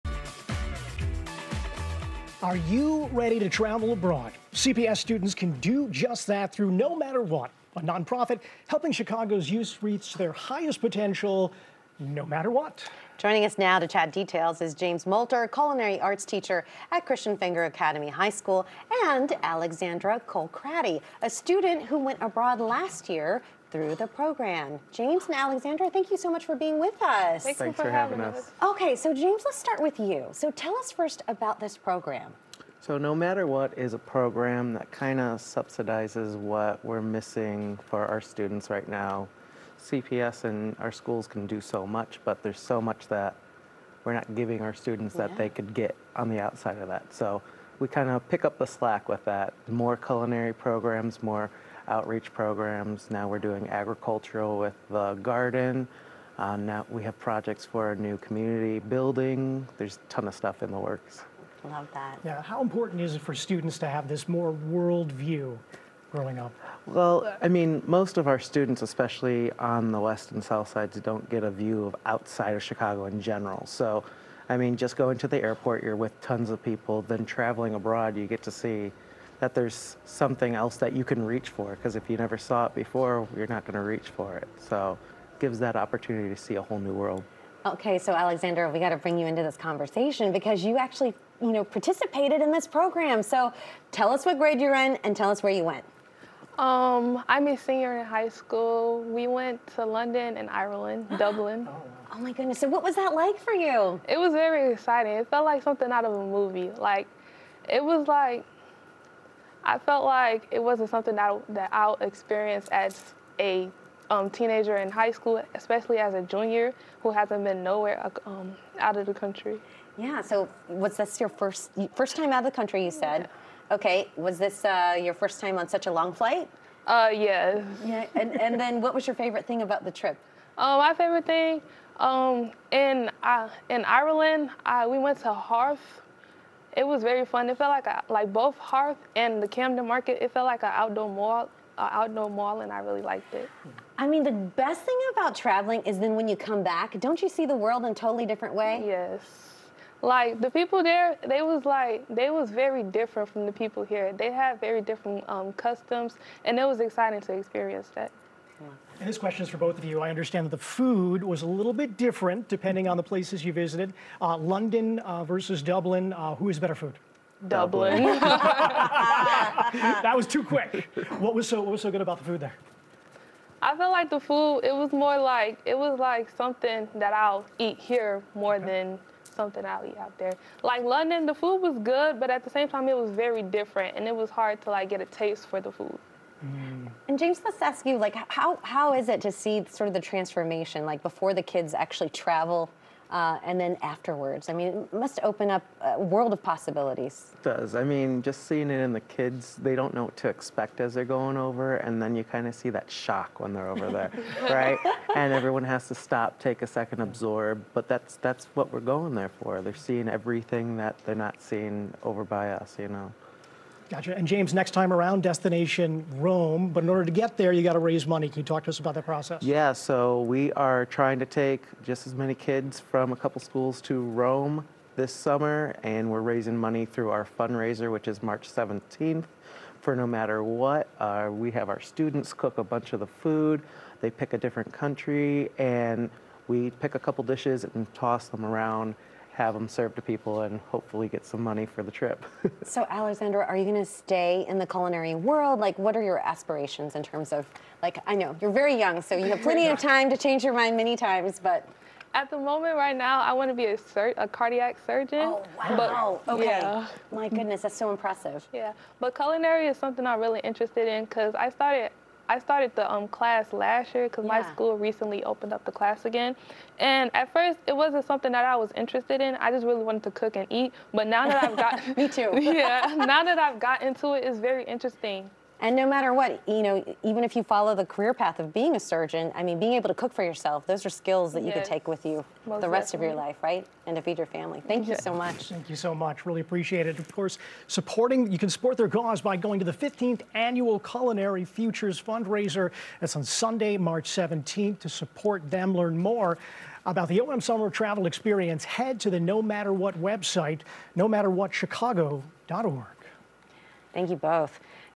Oliver Malcolm Summer Travel Experience For CPS Students
Are you ready to travel to Rome?
CPS students can do just that through "No Matter What" -- a nonprofit helping Chicago’s youth reach their highest potential -- no matter what! Joining us now to chat details is James Molter - Culinary Arts teacher at Christian Fenger Academy High School and Alexandra Colekraty - a student who went abroad last year through the program.